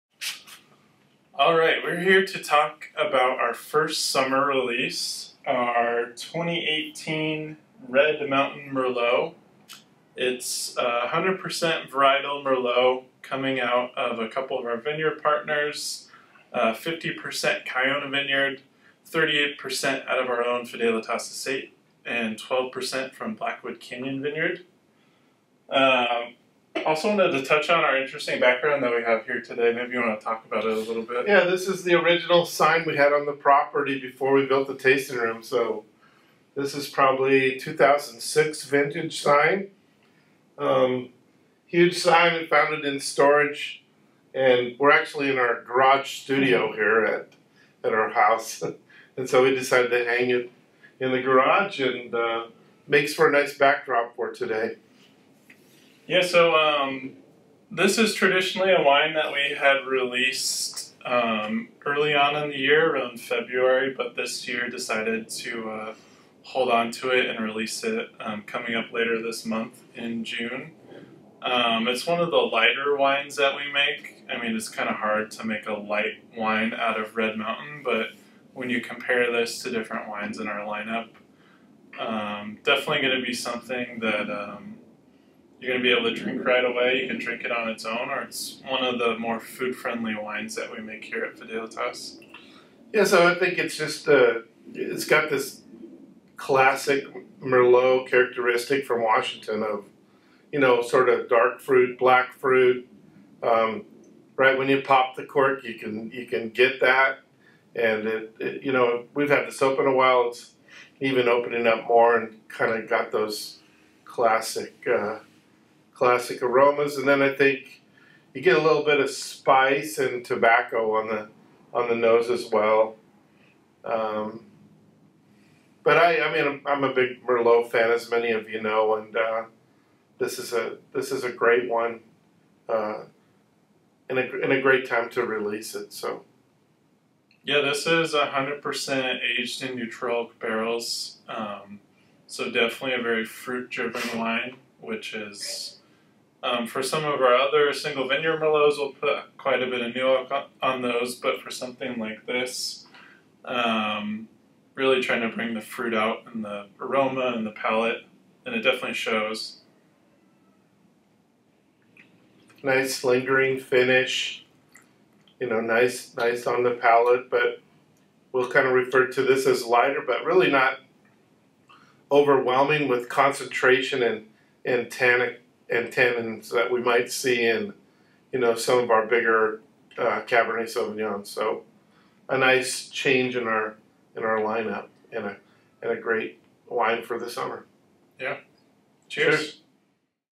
2018 Red Mountain Merlot
572 cases produced

Varietal: 100% Merlot

Vineyard: 50% Kiona Estate Vineyard, 38% Fidelitas Estate Vineyard, 12% Blackwood Canyon Vineyard

Aging: 100% Neutral Oak

Oak Fermentation: 42% 2T Open Top Tank, 21% Roller Barrels